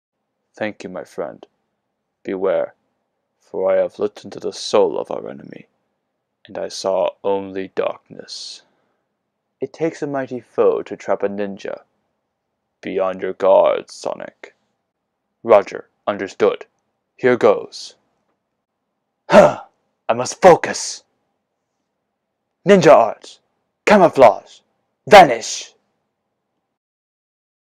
Espio Audition for Knuckles The Echidna
My Espio audition for Knuckles the Echidna. I'm attempting a Troy Baker Espio. I also have a Bill Corkery version of Espio in my Sonic Character Demo Reel video. Thank you and I hope you like it.

All of the following lines are from Sonic Generations